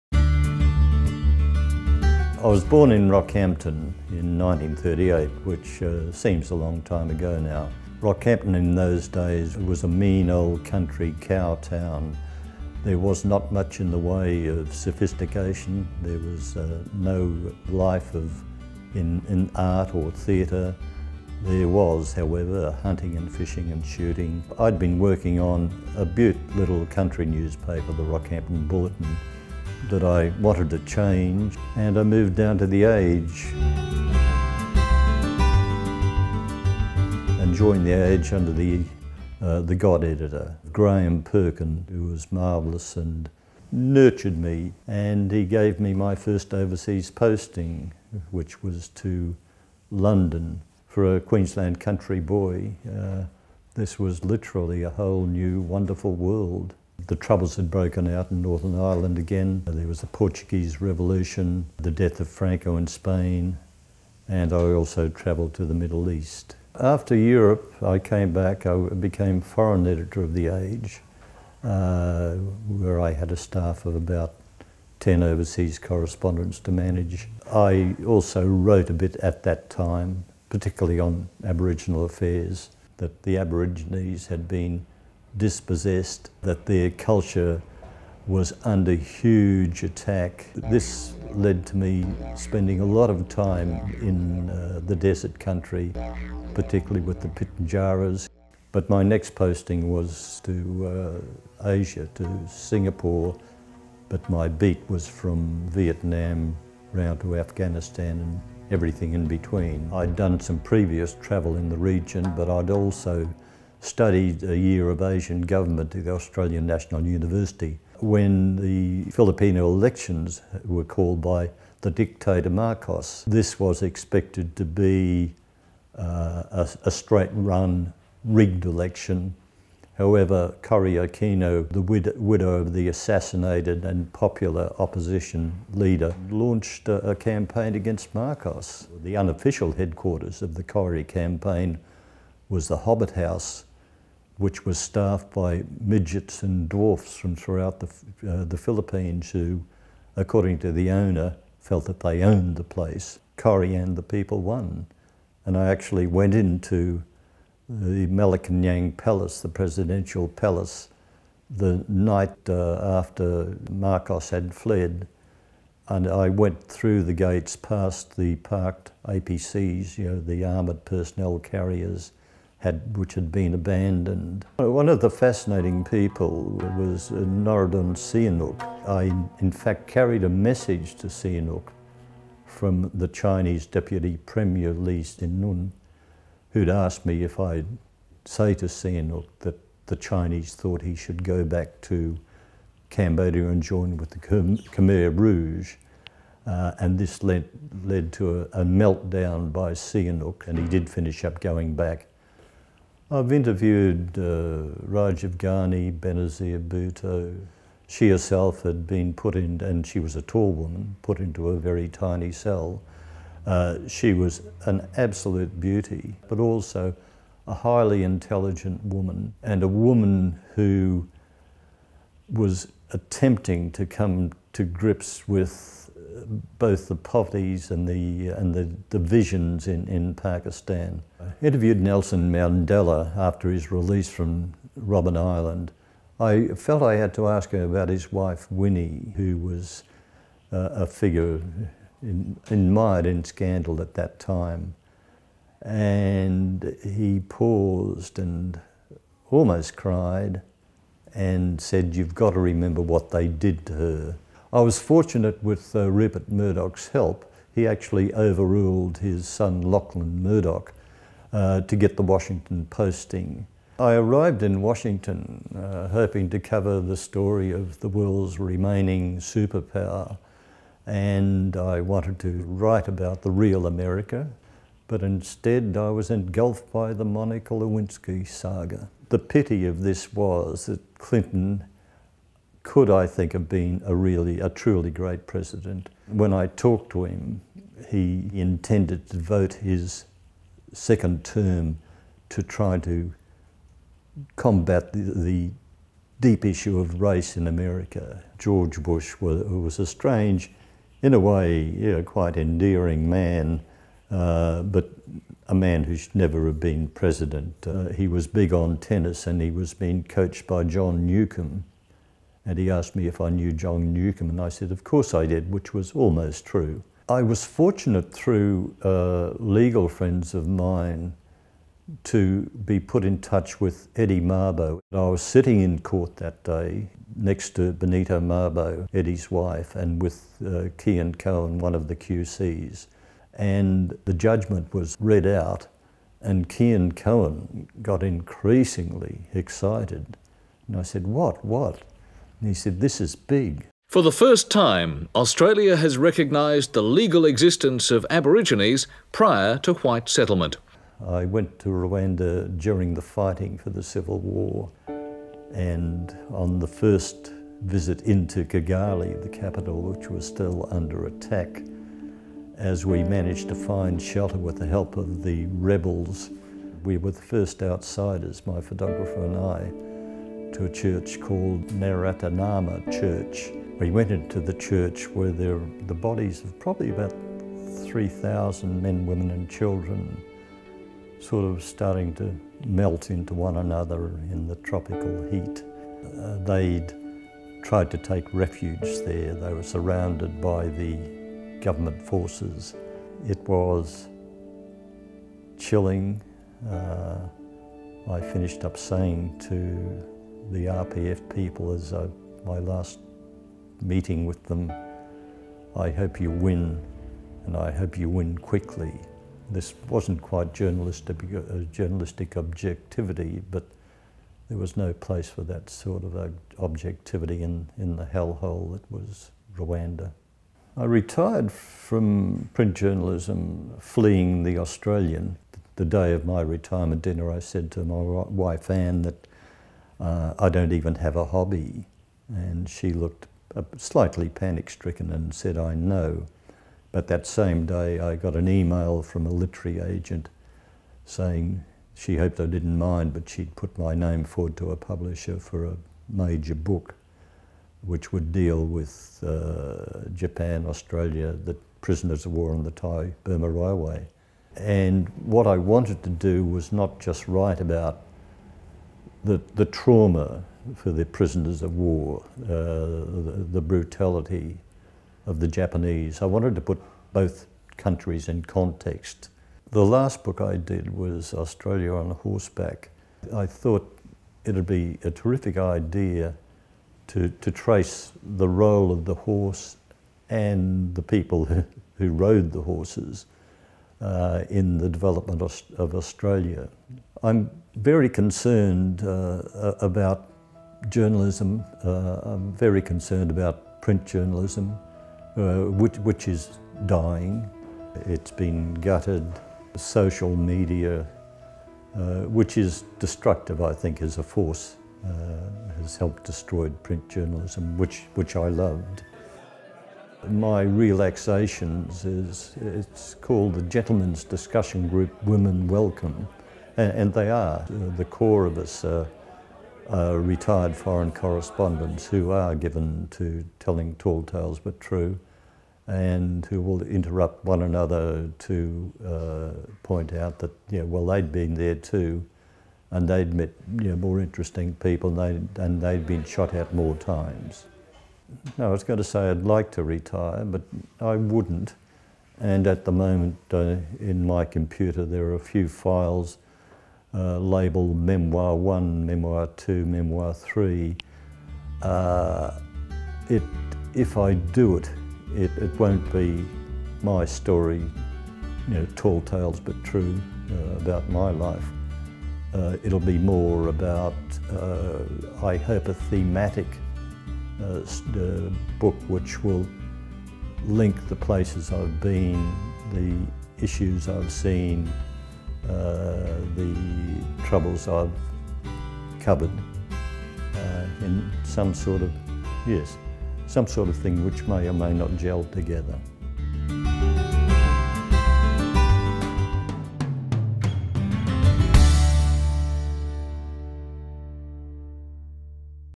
The Cameron Forbes Story
A short Video History of well-known Australian writer and journalist Cameron Forbes.